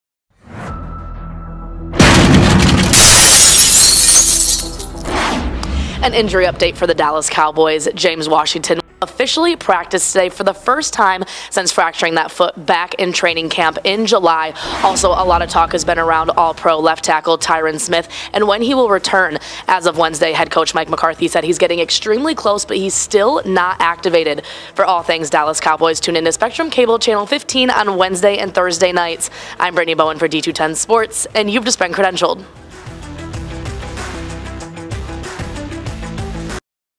Dallas Cowboys Have Officially Started The 21-Day Window To Activate WR James Washington.
FRISCO, TX - The Dallas Cowboys have officially started the 21-day window to activate WR James Washington. Washington will have his first practice with the Cowboys since fracturing his foot during the first padded practice in training camp back in July.

Washington signed as a free-agent during the offseason but has yet to play any games with the Cowboys due to the foot fracture. Washington previously played for the Stealers for four years and had 114 catches for 1,1629 yards receiving for 11 touchdowns.

Dallas’s roster was already full as of Tuesday morning and with space needed to be created for James Washington and Tyron Smith, the Cowboys waived DE Tarell Basham on Tuesday morning. Basham played every game last season and had 627 defensive snaps, which was most amongst any other Dallas defensive line in 2021. This year Basham has only played two games.

According to Head Coach Mike McCarthy the decision to waive Basham was more about spots on the roster than anything else. Dallas is solid in the DE department and has good depth there. Where the Cowboys need to see more is at WR. Washington will bring another wideout option to Dallas while they figure out what the plan will be with Odell Beckham Jr.

There’s been a lot of talk about if the Cowboys are interested in Beckham Jr. and the answer is yes. Jerry Jones and the Cowboys are expected to meet with him this upcoming Monday for discussion.

“Yes we are,” Jones tells 1053thefan about still planning on meeting with Beckham Jr. “We may have intermittent conversations but our anticipation is to meet as soon as Monday.”

Beckham Jr. has announced he is still interested in the Bills and the Giants so having him as another receiver on this roster is up in the air as of right now. If Dallas can not secure Beckham Jr. Washington will be a good addition to the WR depth chart.